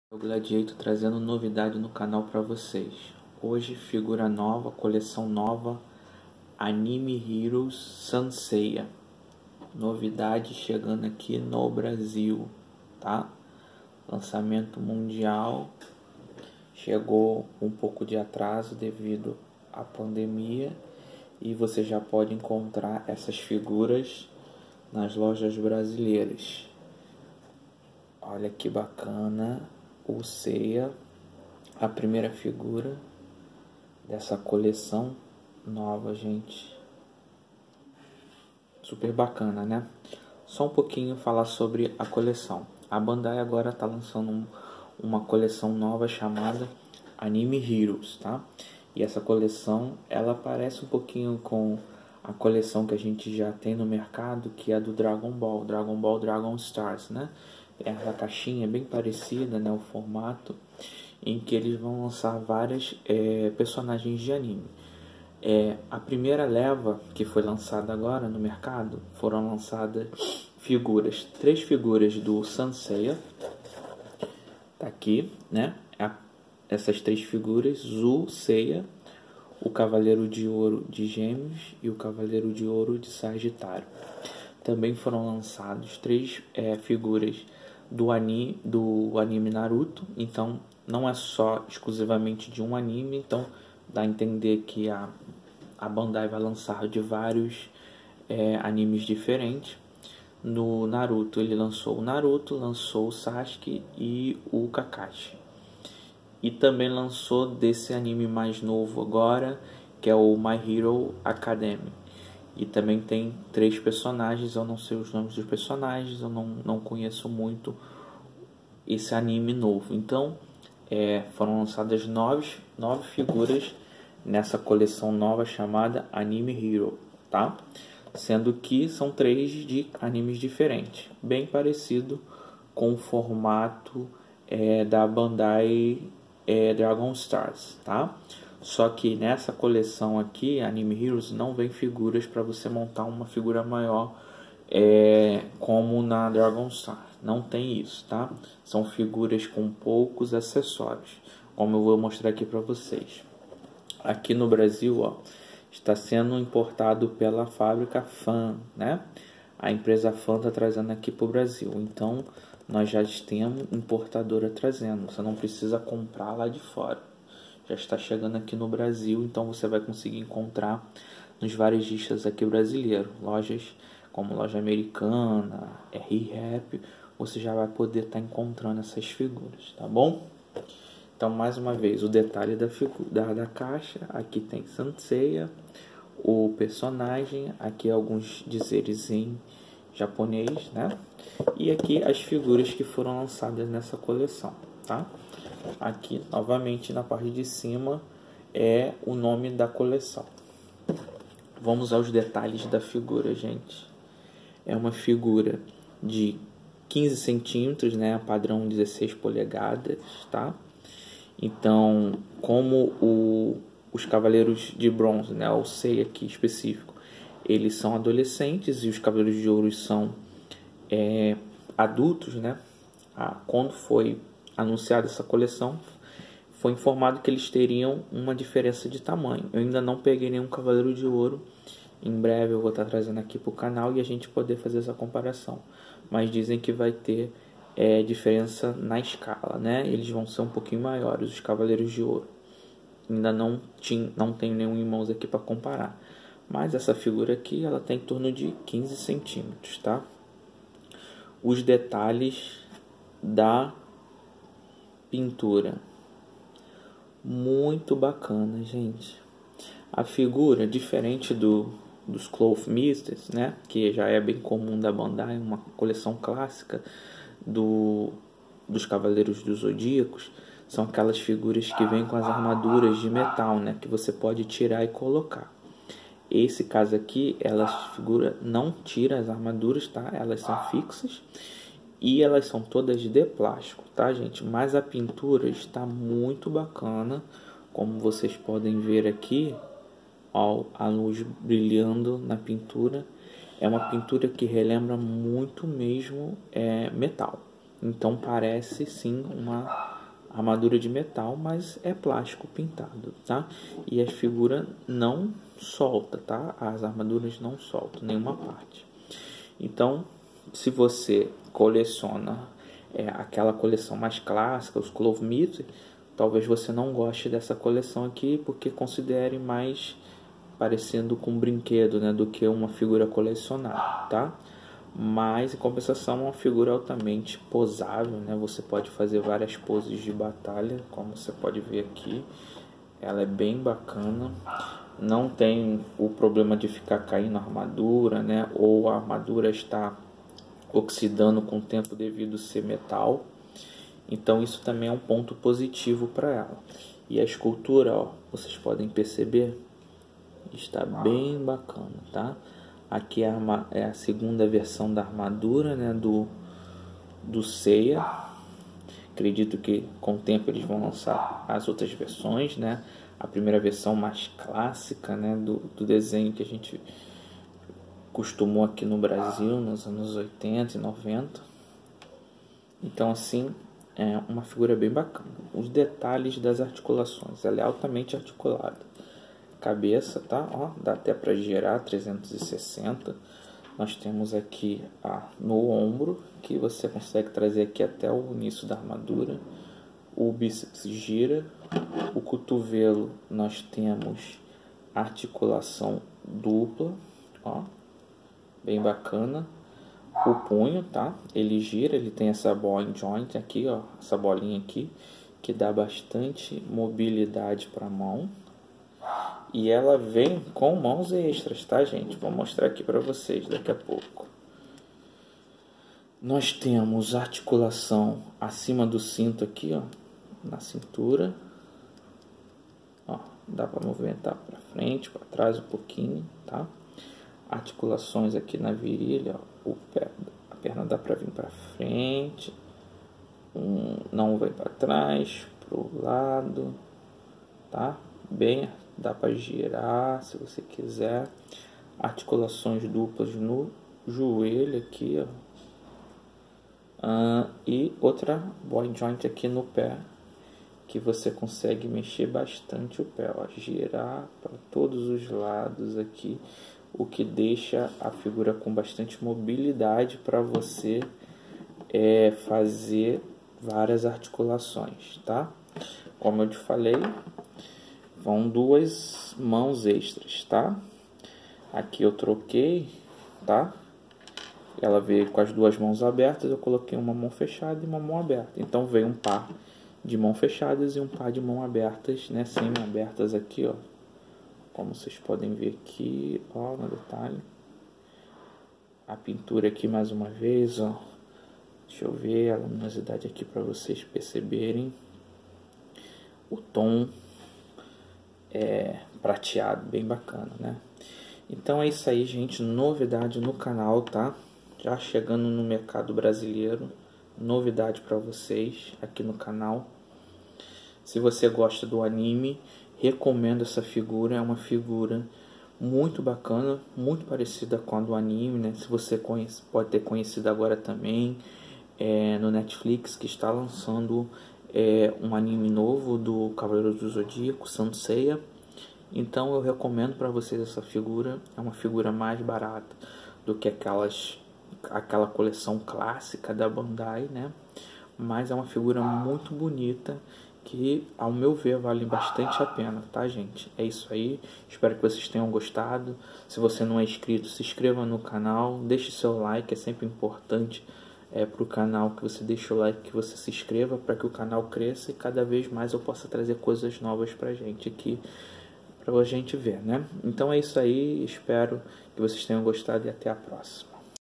Pegasus Seiya - Bandai Anime Heroes Saint Seiya
Este vídeo é um review da figura de ação Pegasus Seiya da fabricante  Bandai, coleção Anime Heroes Saint Seiya